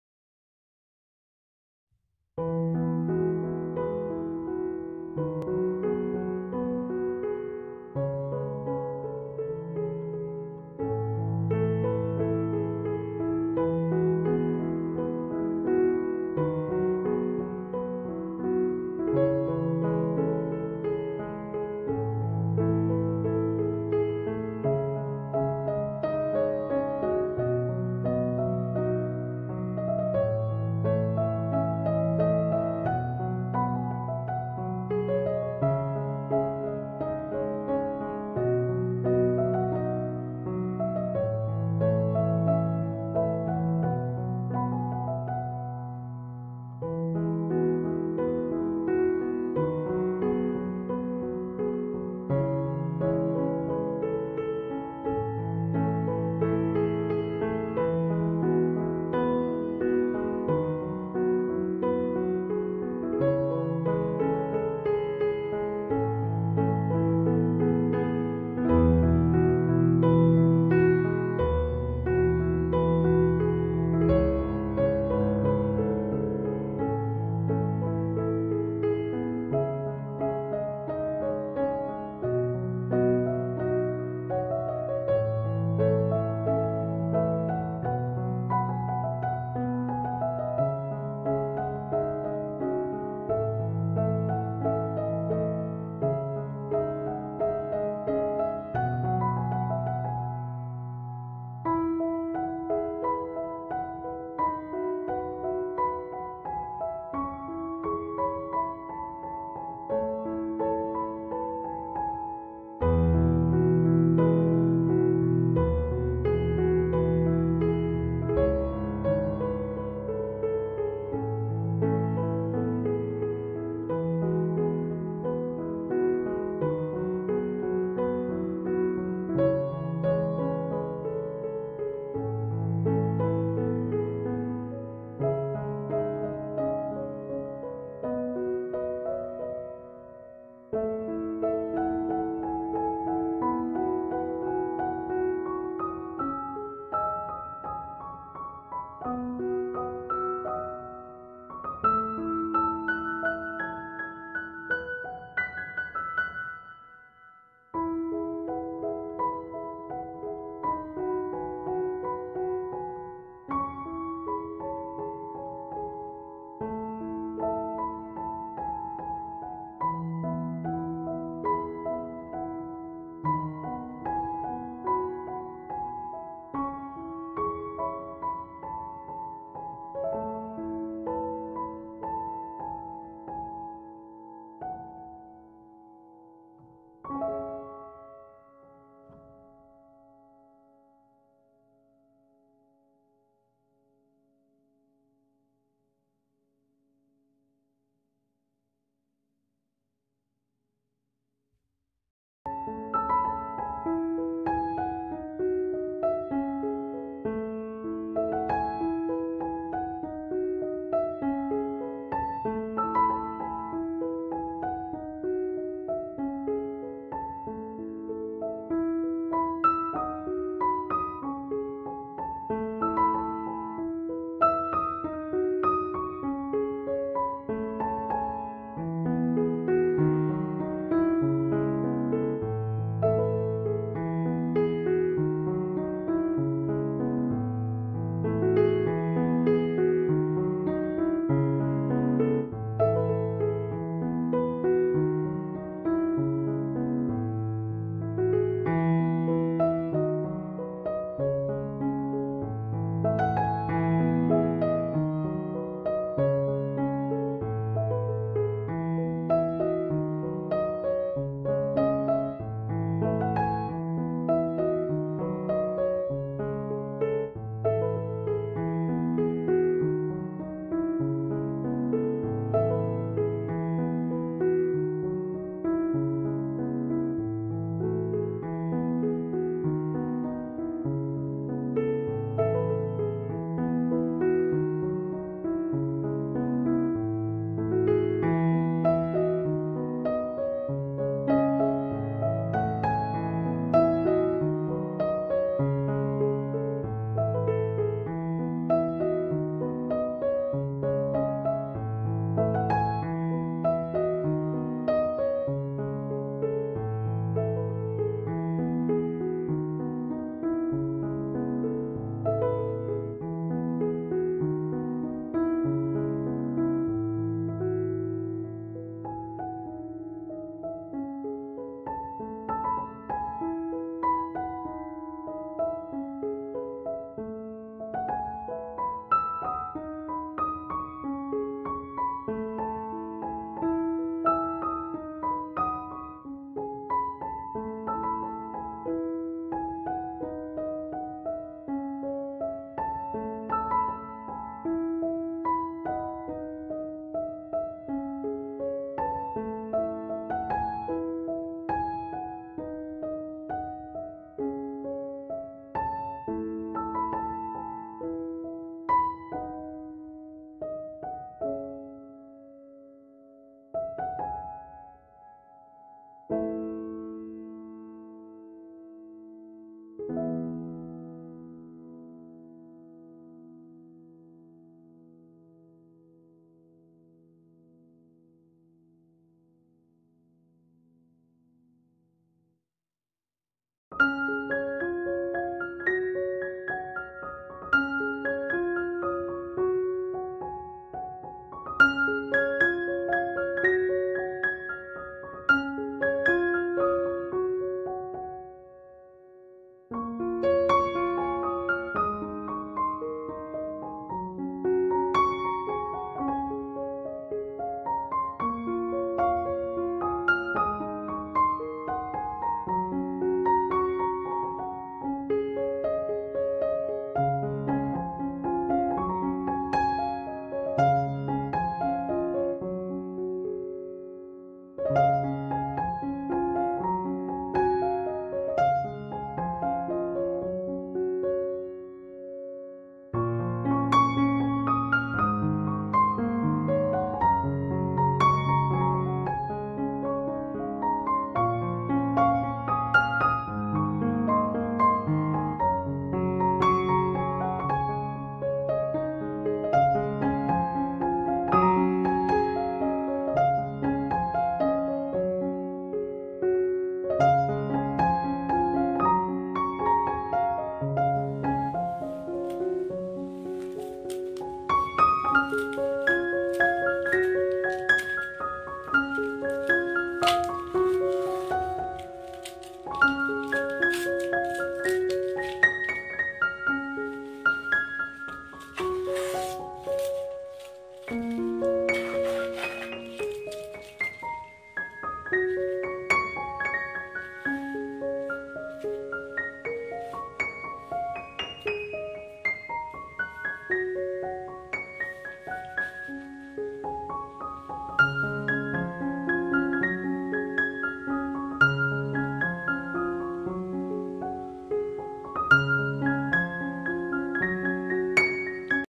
Acrylic Power Lines At Sunset Painting / with Art With zahid / fast motion
If you want to decorate your home wall. Why you west your time i will give you ideas. Very easy tutorial video.